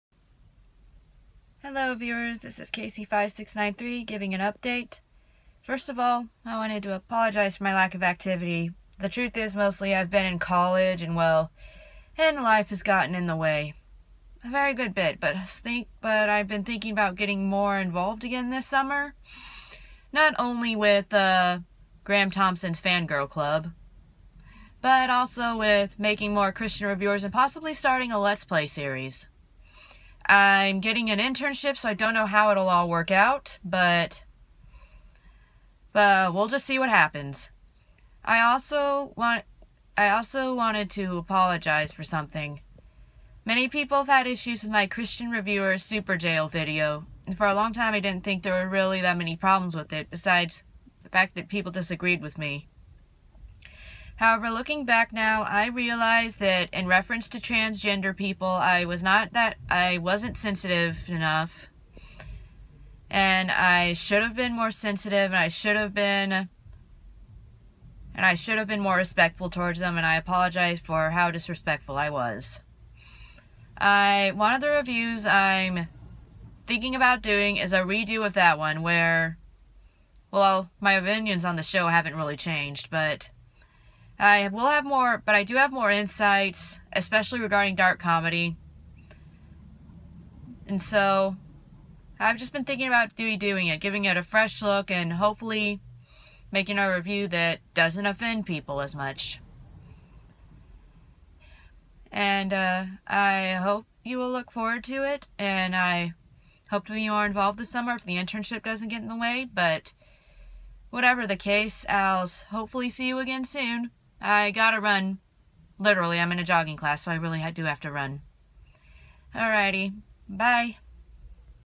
Casey5693 Update
This is an update on my life for my subscribers and the people who were offended by "Superjail!" Review.